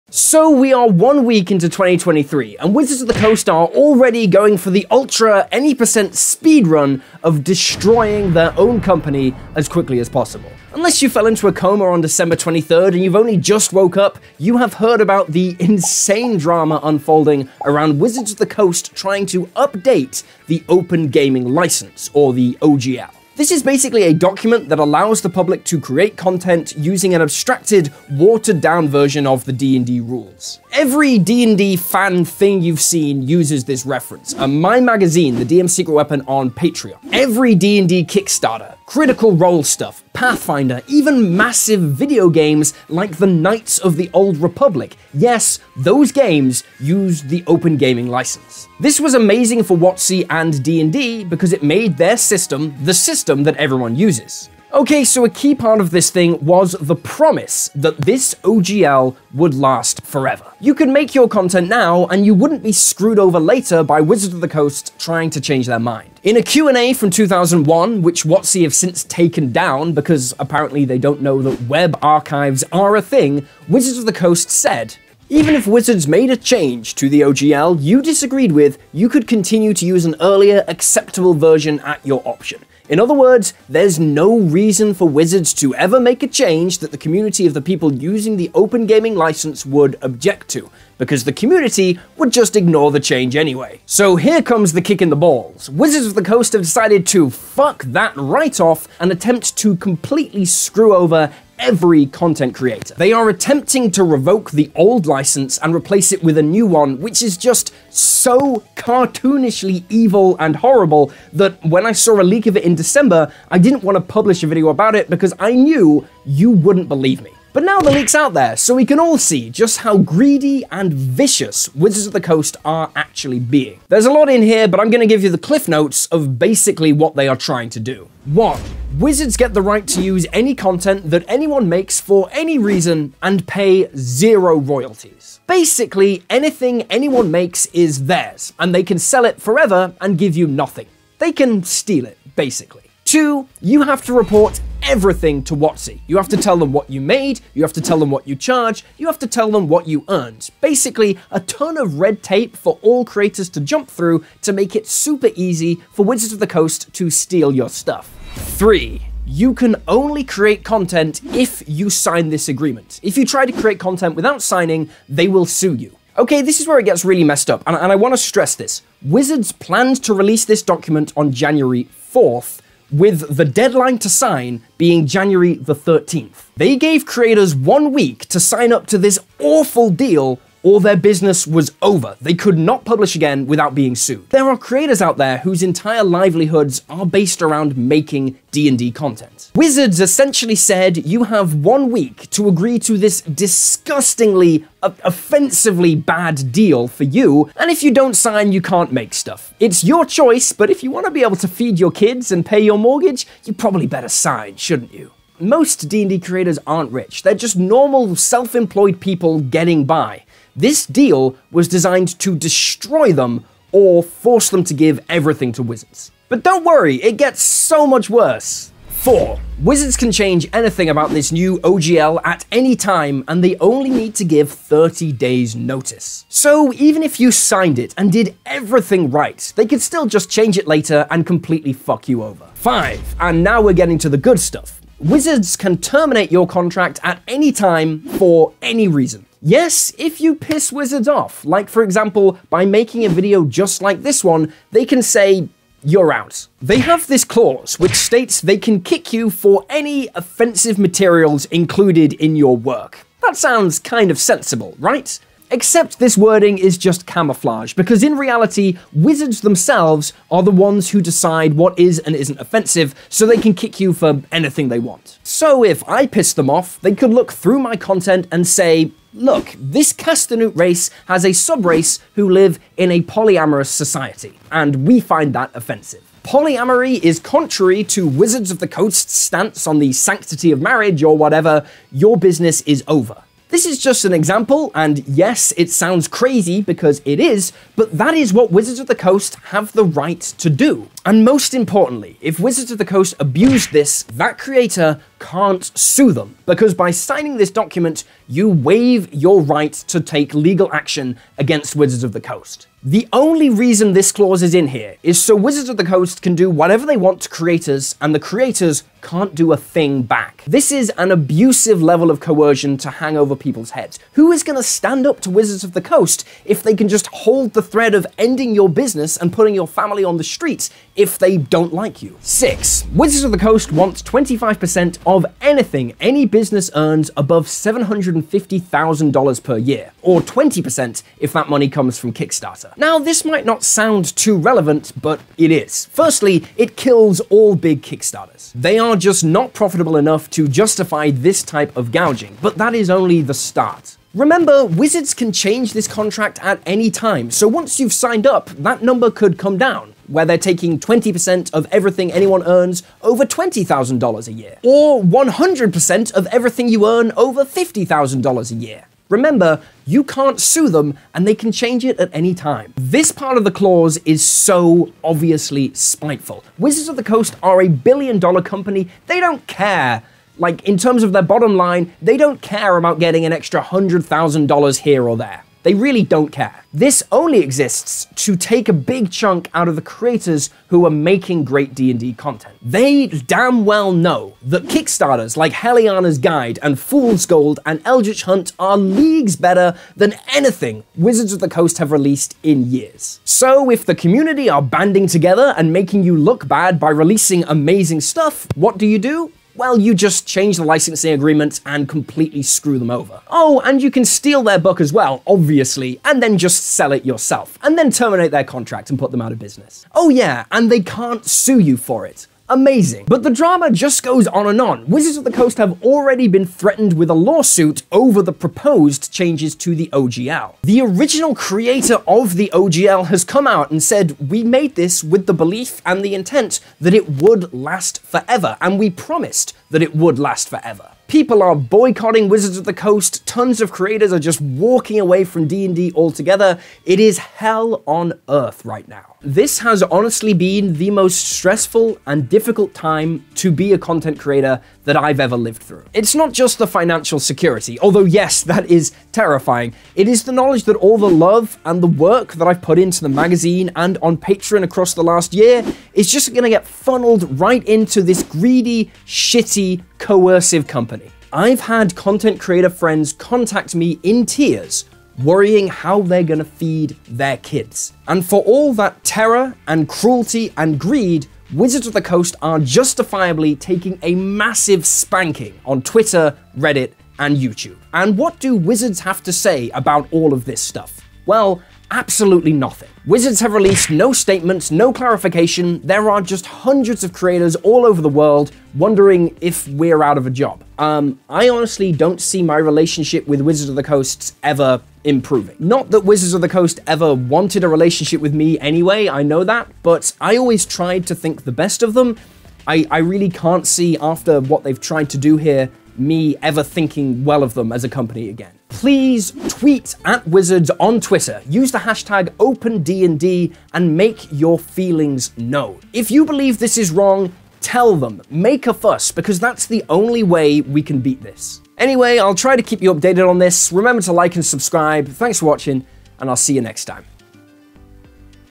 Greed, Lies, and Massive D&D Drama
WotC are destroying this game by revoking the OGL (Open Gaming Licence), using independant creators to light the own funeral pyre.